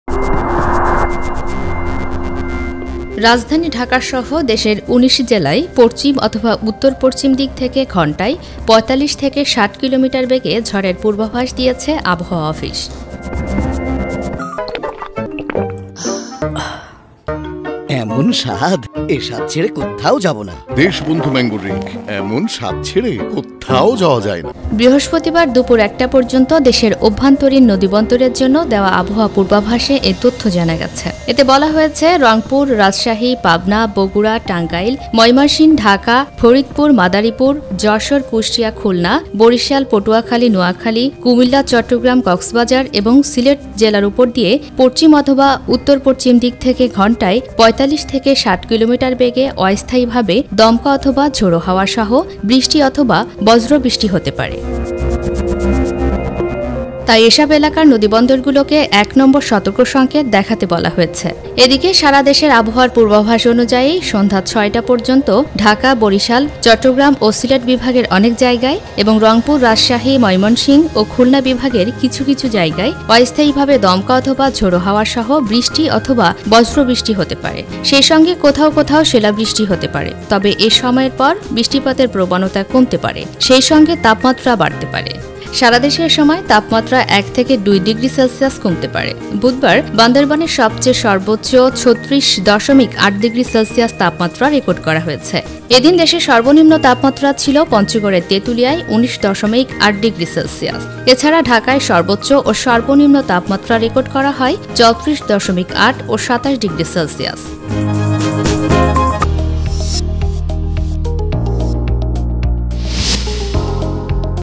ঢাকাসহ ১৯ জেলায় ধেয়ে আসছে তীব্র ঝড়! | দেশবন্ধু টিভি | DeshbandhuTV
রাজধানী ঢাকাসহ দেশের ১৯ জেলায় পশ্চিম অথবা উত্তর-পশ্চিম দিক থেকে ঘণ্টায় ৪৫ থেকে ৬০ কিলোমিটার বেগে ঝড়ের পূর্বাভাস দিয়েছে আবহাওয়া অফিস।

বৃহস্পতিবার (৪ মে) দুপুর ১টা পর্যন্ত দেশের অভ্যন্তরীণ নদীবন্দরের জন্য দেয়া আবহাওয়ার পূর্বাভাসে এ তথ্য জানা গেছে।

▶🔶 Search Me in google : deshbandhu TV